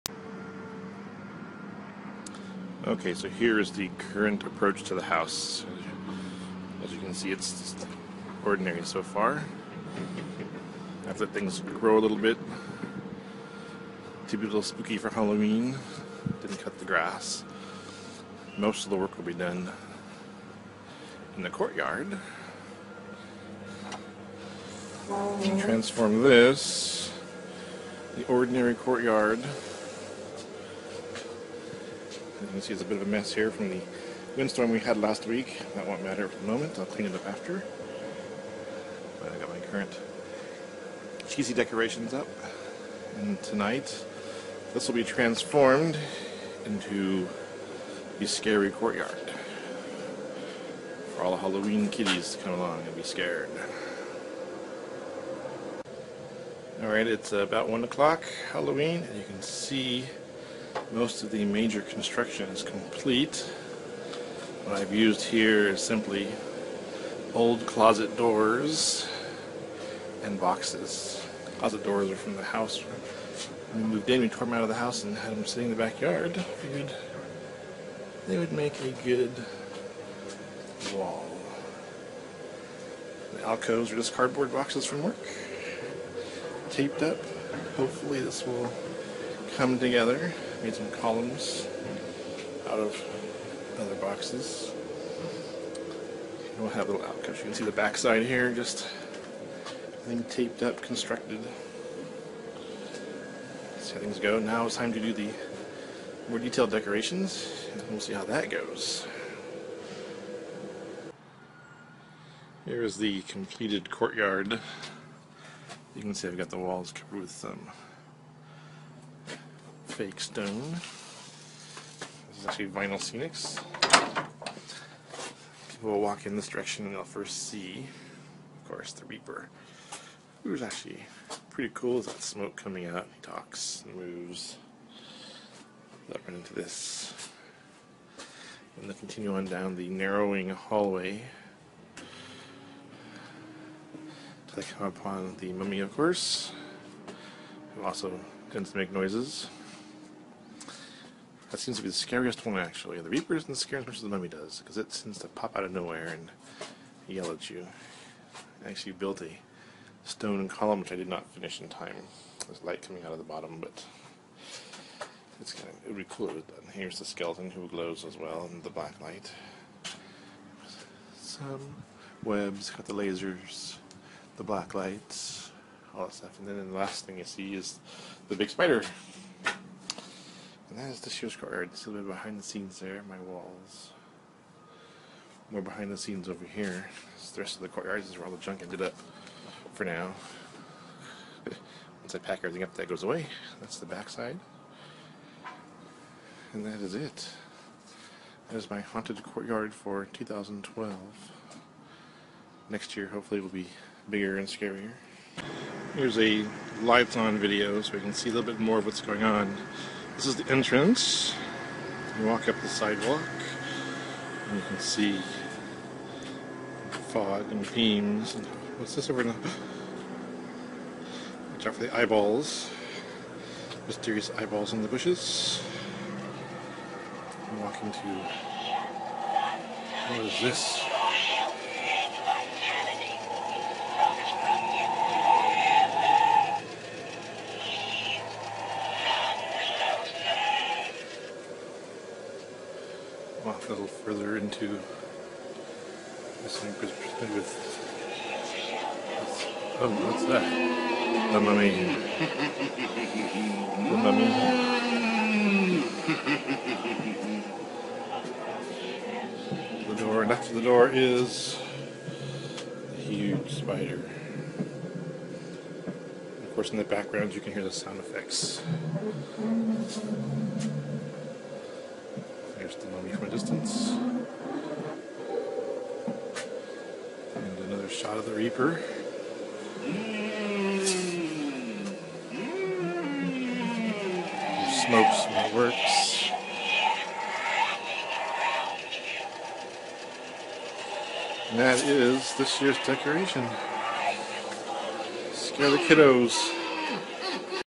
haunted Courtyard 2012
A walk through of my 2012 Halloween decorations, first year at the new house.